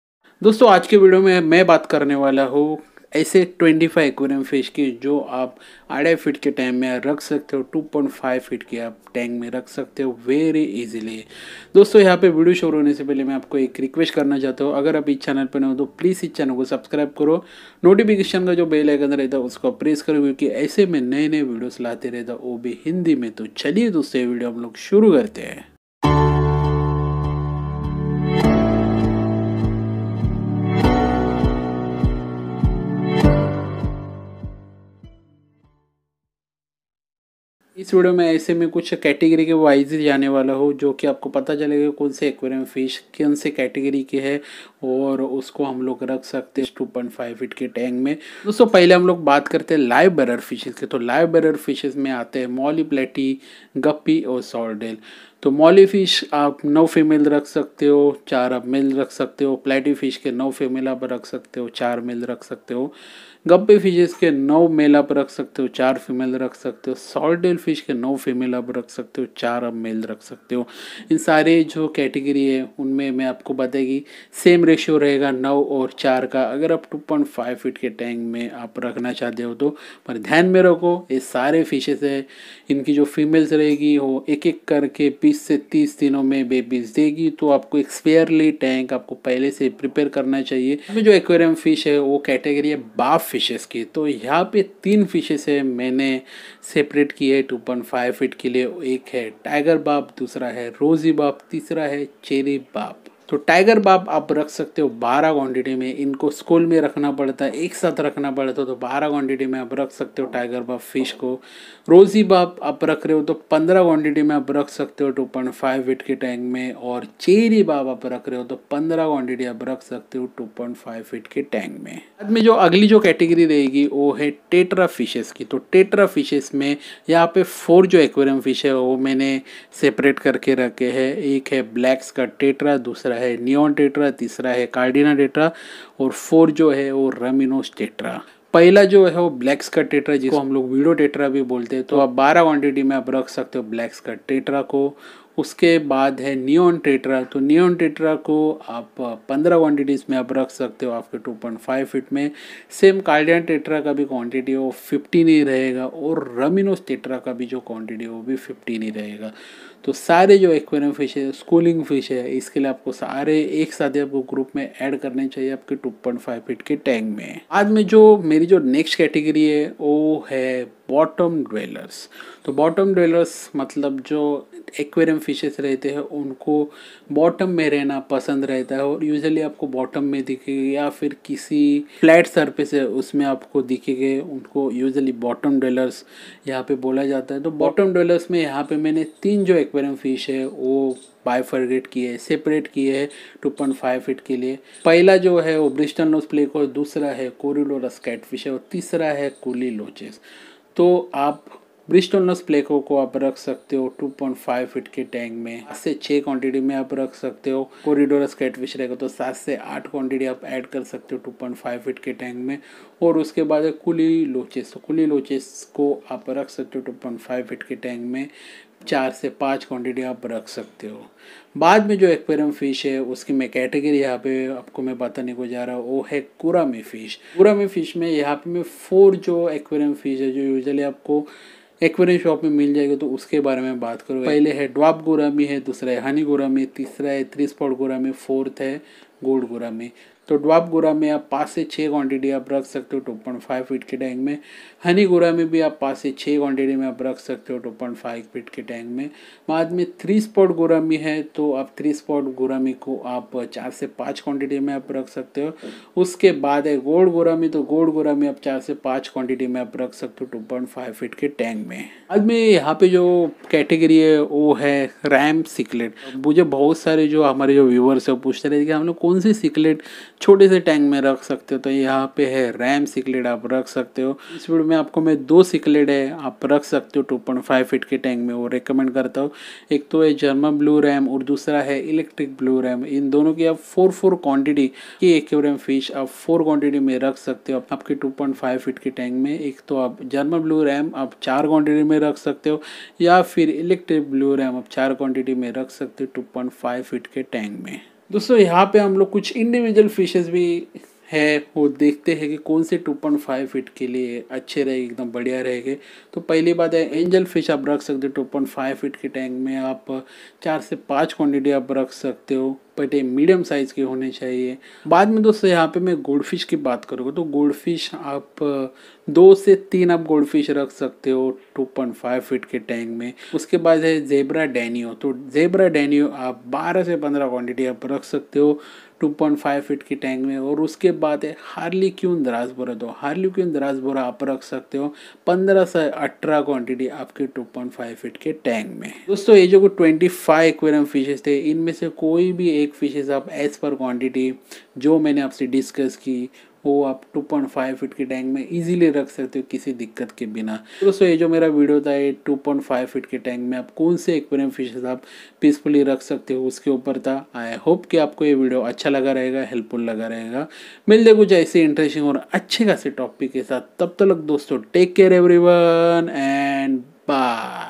Best 25 Aquarium Fish For 2.5 Feet Tank
In this video i am going to share with you best 25 aquarium fish for your 2.5 feet tank

दोस्तो आज के वीडियो में आपको बताने जा रहा हु 25 एक्वेरियम फिश आपके 2.5 फुट के टैंक के लिए

My new channel subscription link

Music credit. ,,,,,,,,,,,